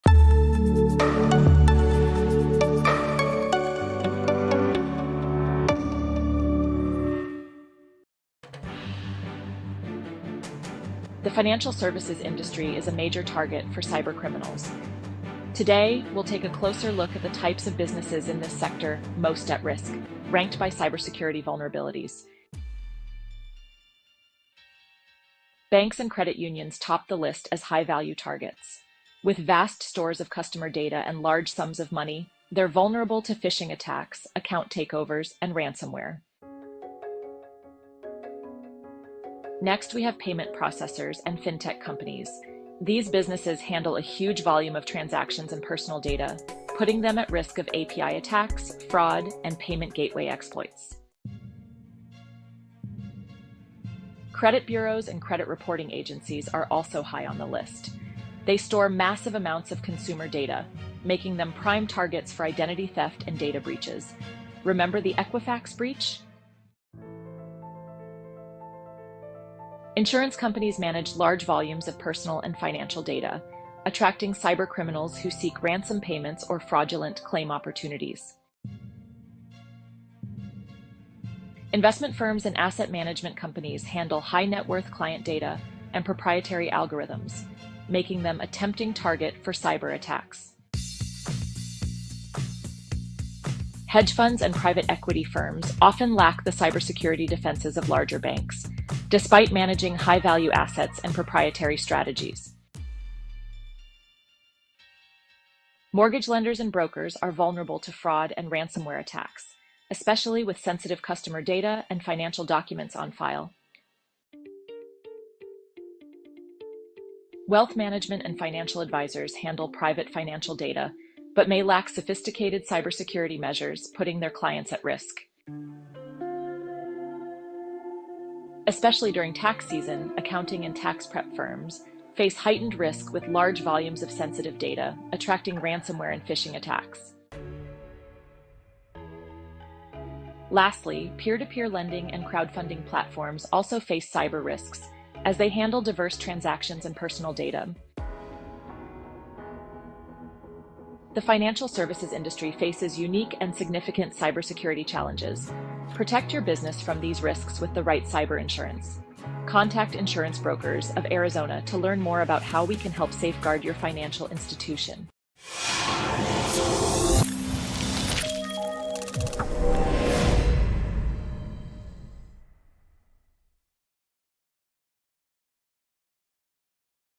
Who Is at Risk in the Financial Services Industry? Cybersecurity, Compliance, and Market Challenges!
The financial services industry operates in a high-stakes environment, facing unique challenges that impact clients, institutions, and the global economy. In this video, we dive into Who is at Risk in the Financial Services Industry and explore the specific risks for each group:

💼 Banks and Financial Institutions: Prime targets for cyber attacks, including data breaches, fraud, and ransomware
👥 Customers and Account Holders: Identity theft and account fraud risks that affect financial well-being
📊 Investors and Shareholders: Market volatility and regulatory changes that impact portfolios and shareholder value
🔍 Compliance and Regulatory Pressure: Why staying compliant is essential and the risks of regulatory fines and penalties
📈 Fintech and Emerging Technologies: The rise of financial technology and its associated cyber risks, data privacy issues, and operational challenges

Whether you're a customer, investor, or financial professional, understanding these risks can help you protect your assets and navigate the financial landscape.

00:00 Introduction
00:23 Banks and Credit Unions
00:38 Payment Processors and FinTech Companies
00:52 Credit Bureaus and Credit Reporting Agencies
01:07 Insurance Companies
01:19 Investment Firms and Asset Management Companies
01:31 Hedge Funds and Private Equity Firms
01:43 Mortgage Lenders and Brokers
01:56 Wealth Management and Financial Advisors
02:07 Accounting and Tax Preparation Firms
02:18 Peer-to-Peer Lending and Crowdfunding Platforms